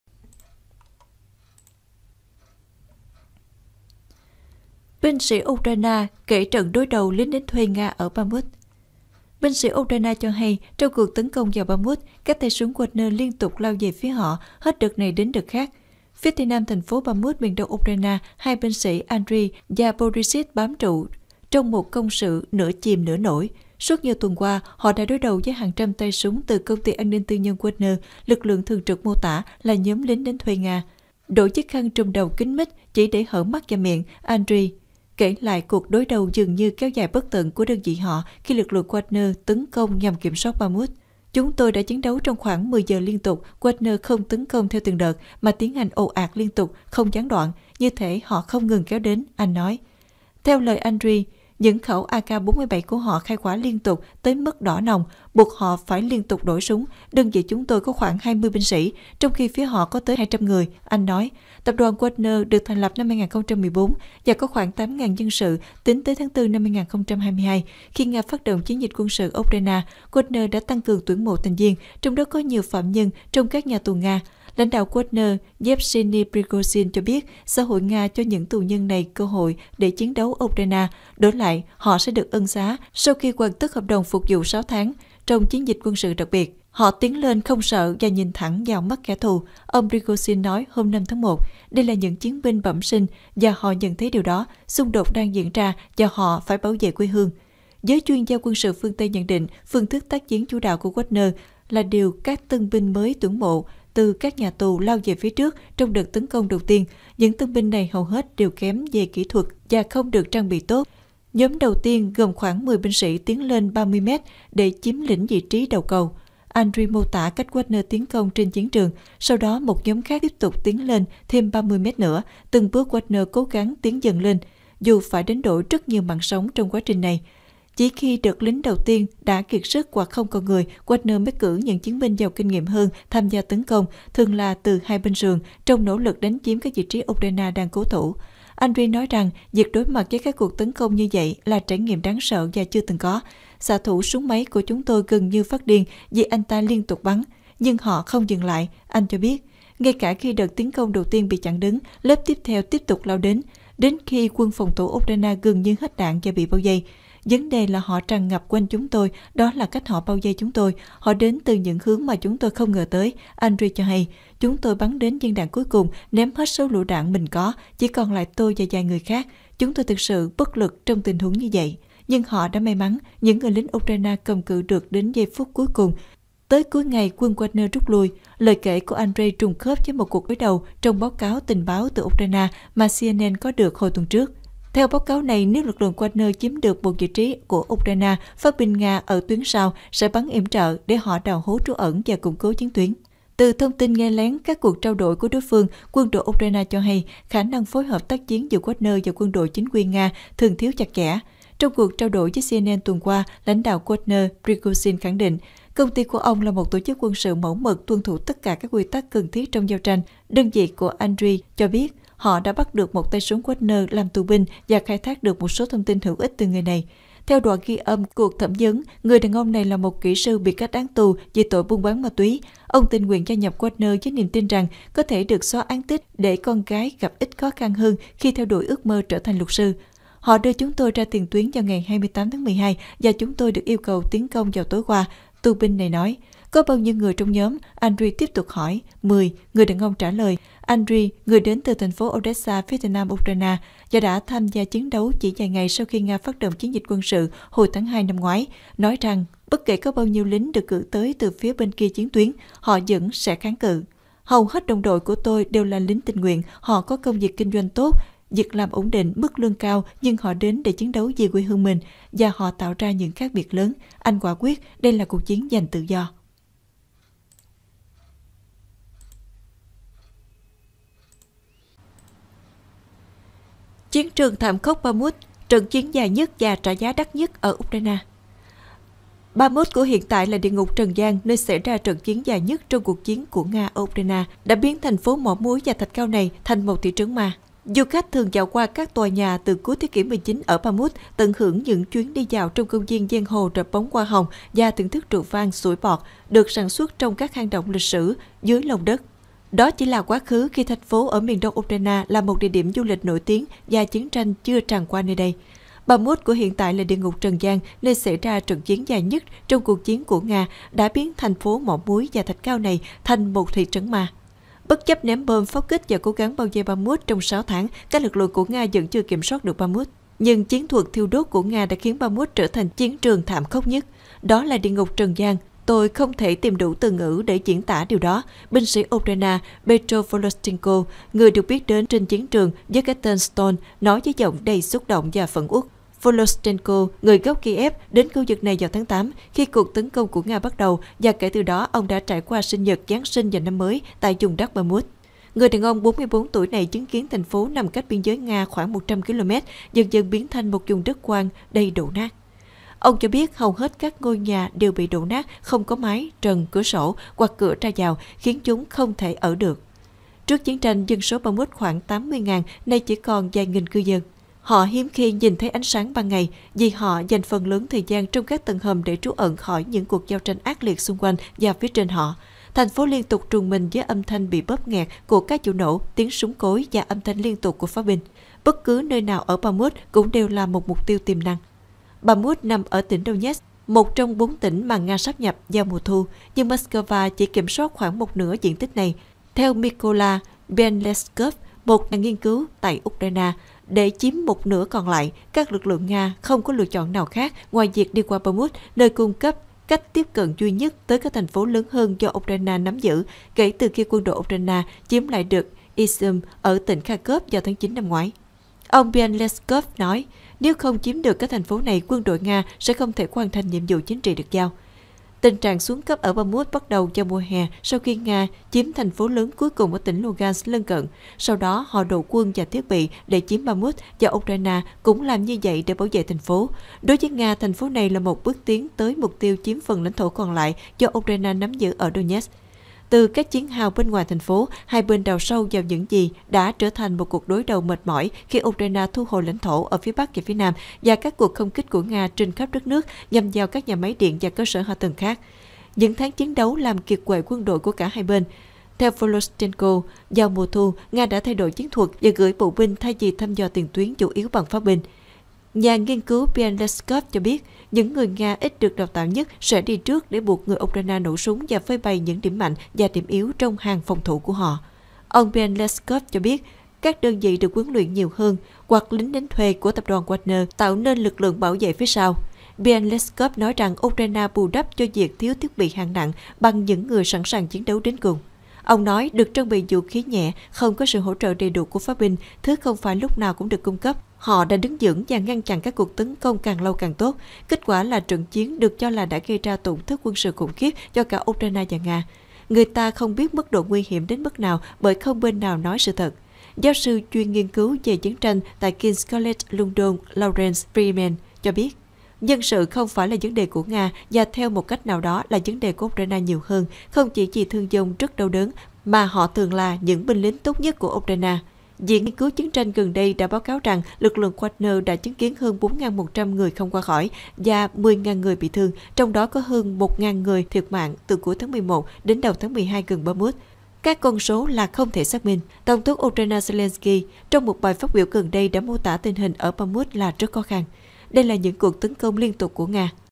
Nga Ukraine tối 3/2: Binh sĩ Ukraine hé lộ trận đối đầu khốc liệt với lính đánh thuê Nga ở Bakhmut
Binh sĩ Ukraine hé lộ trận đối đầu khốc liệt với lính đánh thuê Nga ở Bakhmut
Binh sĩ Ukraine cho hay trong cuộc tấn công vào Bakhmut, các tay súng Wagner liên tục lao về phía họ, hết đợt này đến đợt khác.

Phía tây nam thành phố Bakhmut, miền đông Ukraine, hai binh sĩ Andriy và Borisych bám trụ trong một công sự nửa chìm nửa nổi. Suốt nhiều tuần qua, họ đã đối đầu với hàng trăm tay súng từ công ty an ninh tư nhân Wagner, lực lượng thường được mô tả là nhóm "lính đánh thuê" Nga.

Đội chiếc khăn trùm đầu kín mít chỉ để hở mắt và miệng, Andriy kể lại cuộc đối đấu dường như kéo dài bất tận của đơn vị họ, khi lực lượng Wagner tấn công nhằm kiểm soát Bakhmut.
"Chúng tôi đã chiến đấu trong khoảng 10 giờ liên tục. Wagner không tấn công theo từng đợt, mà tiến hành ồ ạt, liên tục, không gián đoạn, như thể họ không ngừng kéo đến", anh nói.

Theo lời Andriy, những khẩu AK-47 của họ khai hỏa liên tục đến mức đỏ nòng, buộc họ phải liên tục đổi súng. "Đơn vị chúng tôi có khoảng 20 binh sĩ, trong khi phía họ có tới 200 người", anh nói.

Tập đoàn Wagner được thành lập năm 2014 và có khoảng 8.000 nhân sự tính tới tháng 4/2022. Khi Nga phát động chiến dịch quân sự ở Ukraine, Wagner đã tăng cường tuyển mộ thành viên, trong đó có nhiều phạm nhân trong các nhà tù Nga.

Lãnh đạo Wagner Yevgeny Prigozhin cho biết xã hội Nga cho những tù nhân này cơ hội để chiến đấu ở Ukraine, đổi lại họ sẽ được ân xá sau khi hoàn tất hợp đồng phục vụ 6 tháng trong "chiến dịch quân sự đặc biệt".